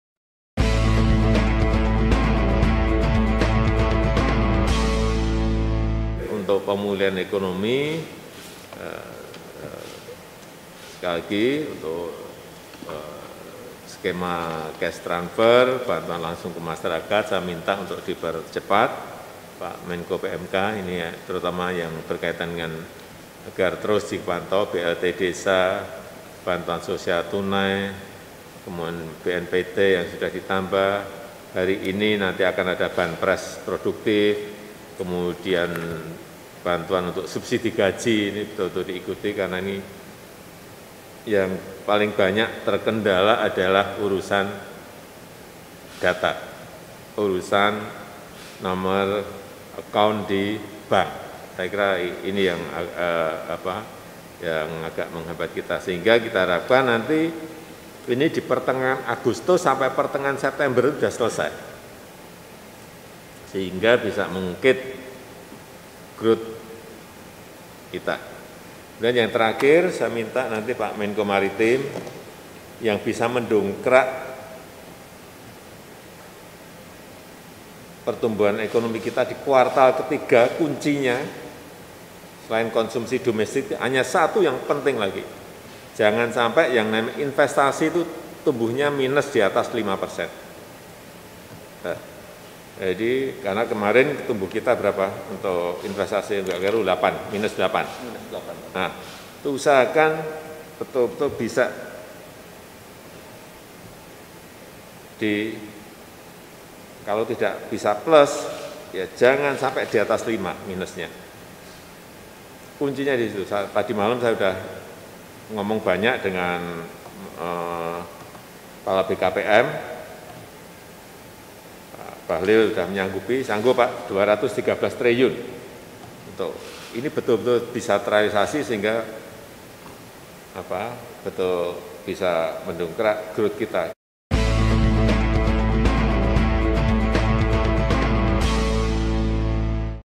Presiden Jokowi Targetkan Data Penyaluran Bansos Selesai Pertengahan September 2020
(Beritadaerah – Nasional) Presiden Joko Widodo mentargetkan, penyelesaian data yang berkaitan dengan penyaluran berbagai bantuan sosial (bansos) selesai paling lambat pertengahan September 2020. Penyaluran bansos diharapkan dapat dilakukan secara merata kepada masyarakat yang terdampak Covid-19, dan dapat selesai pada pertengahan September 2020.

“Urusan data pada nomor account bank nanti selesai pada Agustus sampai pertengahan September,” ujar Presiden Joko Widodo melalui konferensi pers secara virtual di Istana Negara, Jakarta, Senin (24/8/2020).

(Member of Vibiz Media Network)